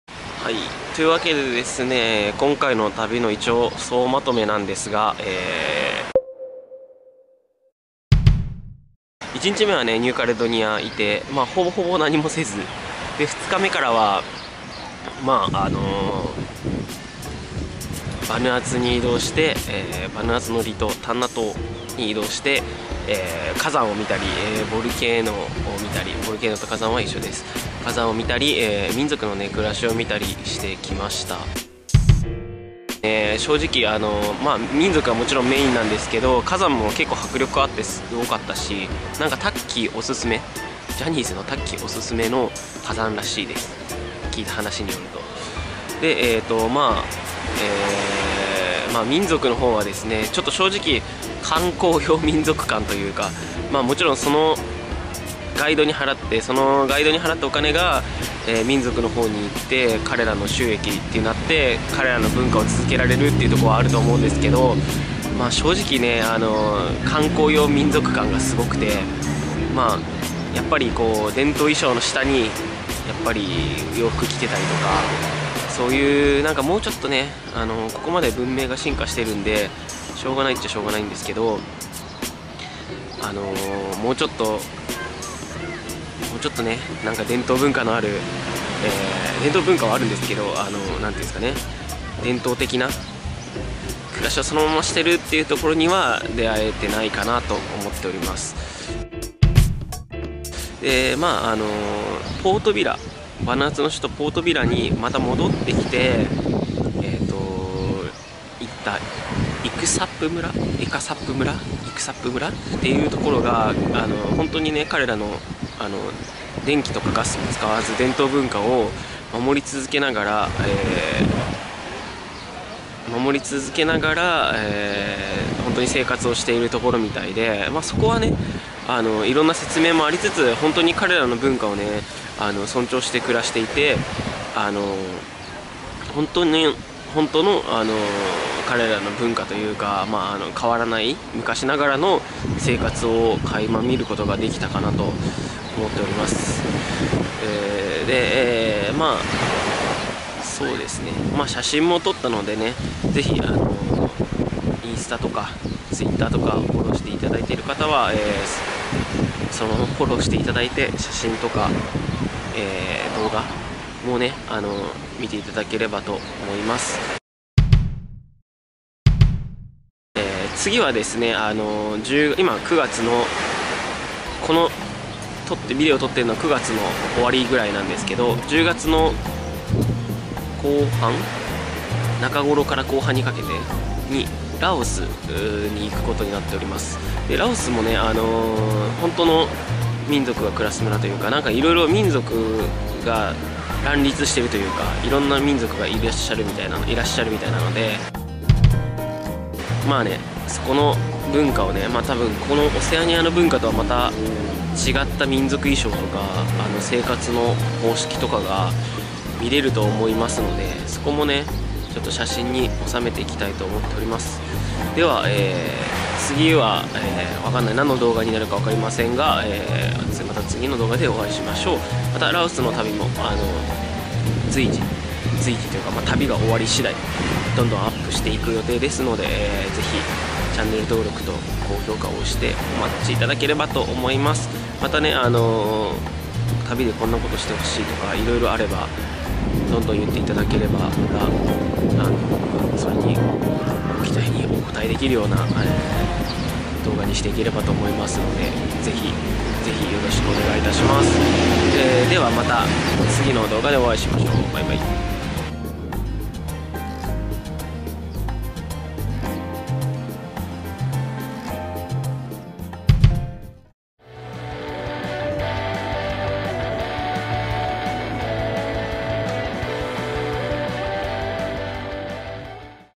【旅動画】ニューカレドニア、バヌアツの旅を総まとめ！編。バヌアツ、ニューカレドニアで少数民族に出会う旅！！！＃１０
【旅動画】世界の少数民族撮影旅〜】ニューカレドニア、バヌアツの旅を総まとめ編#１０〜

今回は総まとめです！ゆるっとバヌアツとニューカレドニア の印象を語っております。

それにしても安全な国だったなあ
犯罪がないことで有名らしいですよ